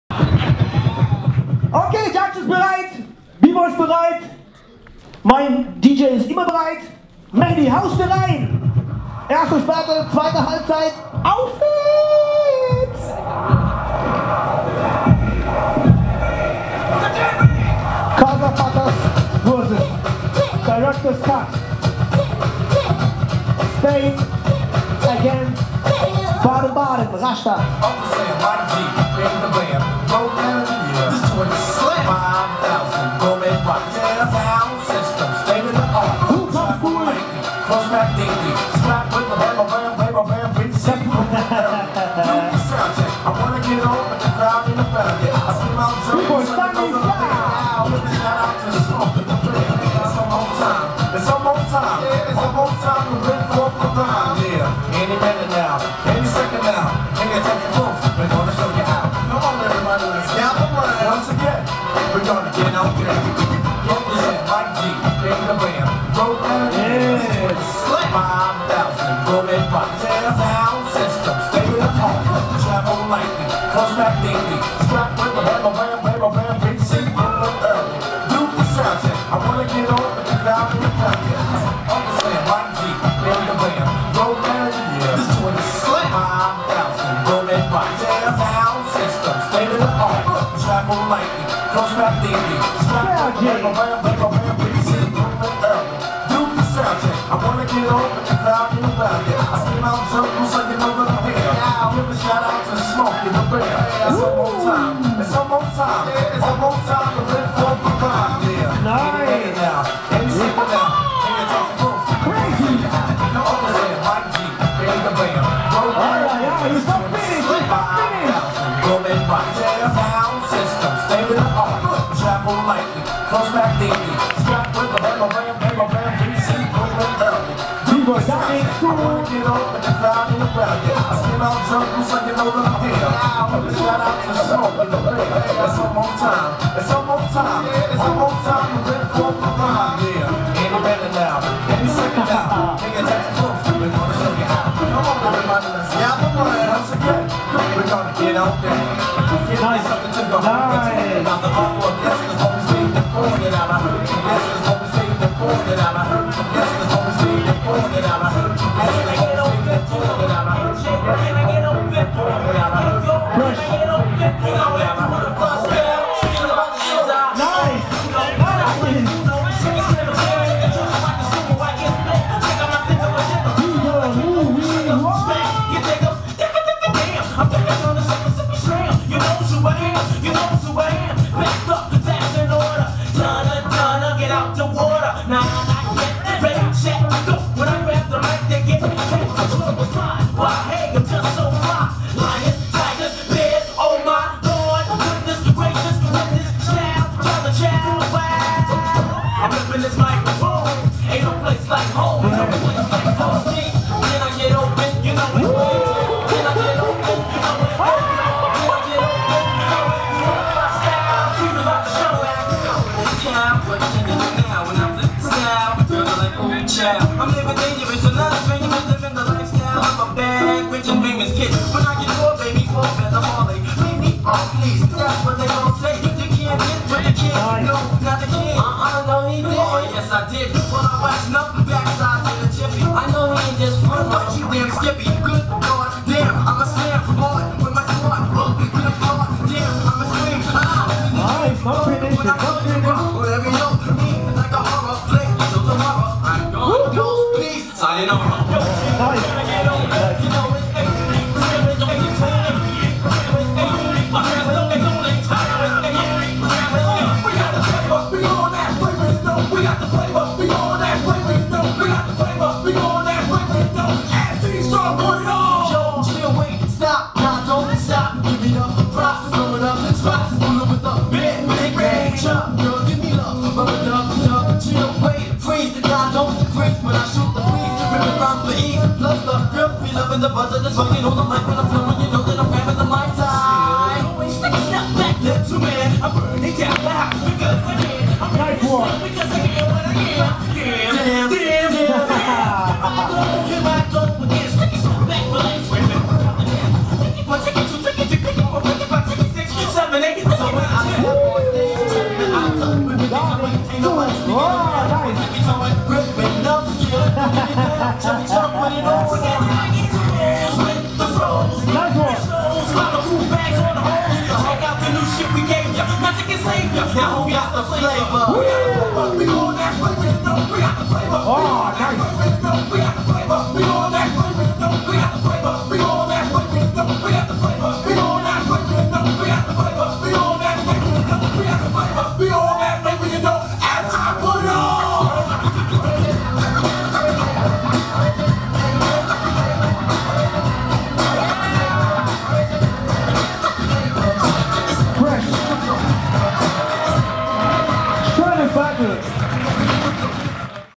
Scenario Battle Castafuckaz vs Directors Cut
Scenario battle
Top 8

Winner: Castafuckaz

Tempel Kulturzentrum Karlsruhe
Combo Hip-Hop Kulturzentrum